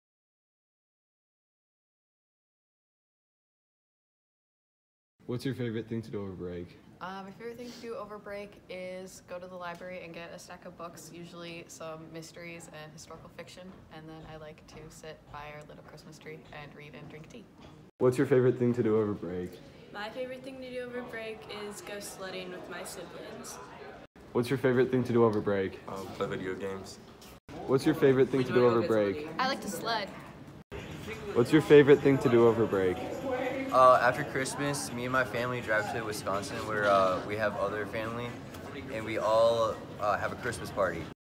[SPARTAN ON THE STREET] What is your favorite activity to do over break?
Upper School students share their favorite activities to do over Winter Break.